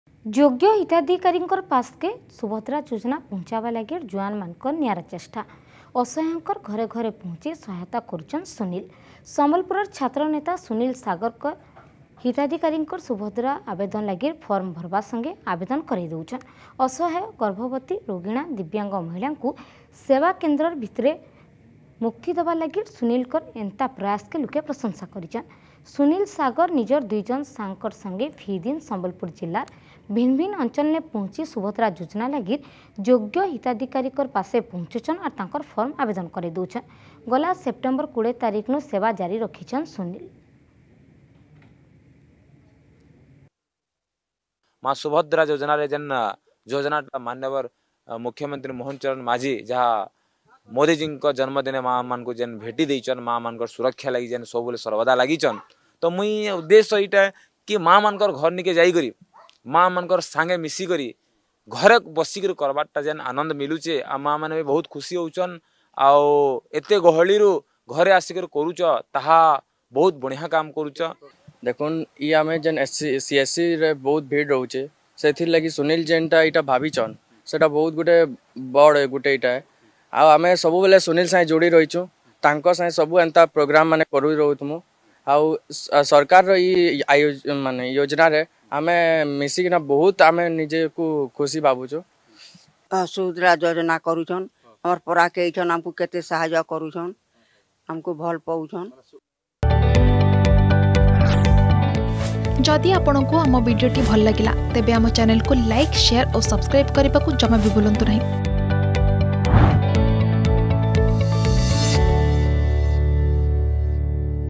ସୁଭଦ୍ରା ଯୁଜନାର୍ ସାରଥୀ ସୁନିଲ୍ ସାଗର୍ | Subhadra Yojana
ସୁଭଦ୍ରା ଯୁଜନାର୍ ସାରଥୀ ସମ୍ବଲପୁରର୍ ଛାତ୍ରନେତା ସୁନିଲ୍ ସାଗର୍ । ଘର୍ ଘର୍ ଯାଇ ହିତାଧିକାରୀକଁର୍ ଭରୁଛନ୍ ସୁଭଦ୍ରା ଫର୍ମ । ଅସହାୟ, ଗର୍ଭବତୀ, ରୋଗୀଣା, ଦିବ୍ୟାଙ୍ଗ ମହିଳାକୁଂ କରୁଛନ୍ ସାହେତା ।